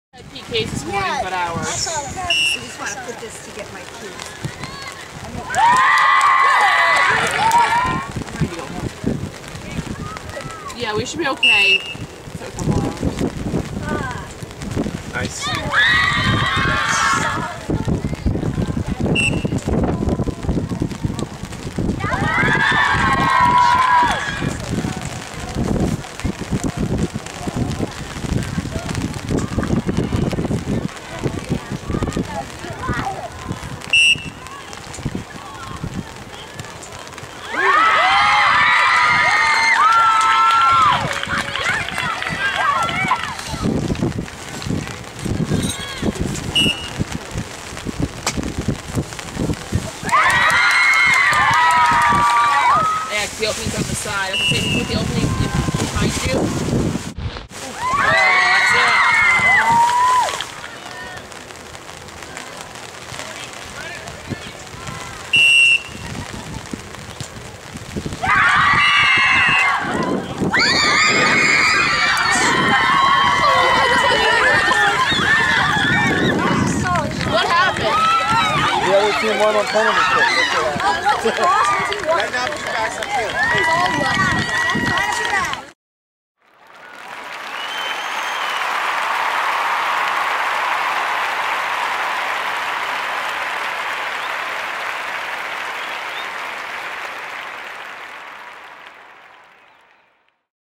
Hershey_2013_Finals_PKs
MSC Stallions 98 vs VYS Thunder Gold play to a scoreless draw after regulation and two five-minute overtime periods.